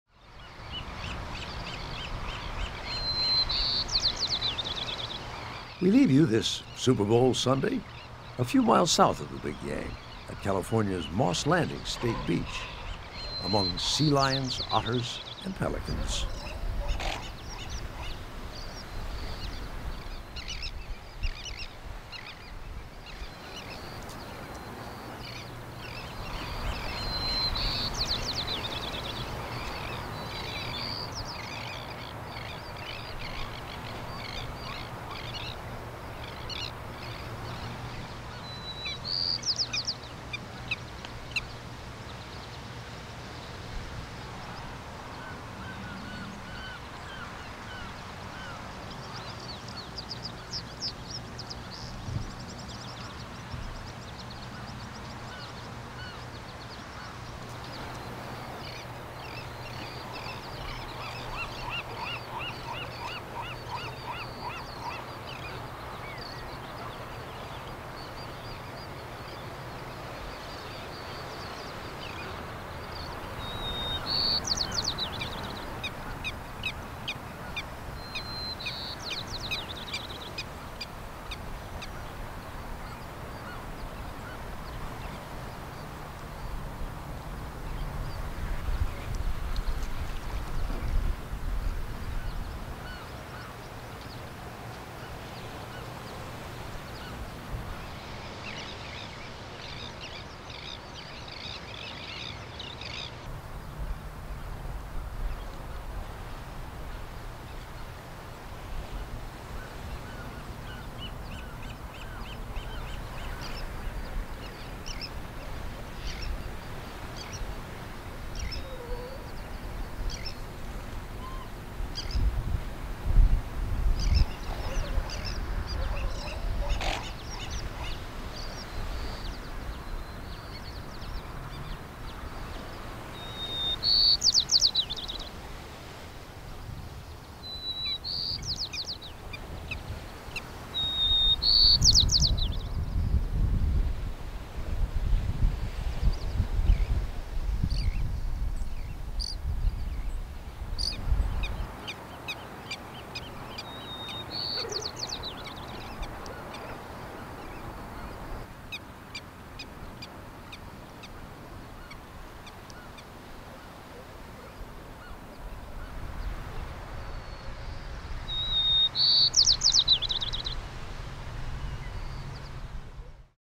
Nature: California's Moss Landing State Beach
We leave you this Super Bowl Sunday a few miles south of the big game, at California's Moss Landing State Beach.  No Panthers and Broncos here ... just sea lions, otters and pelicans. Videography: Scot Miller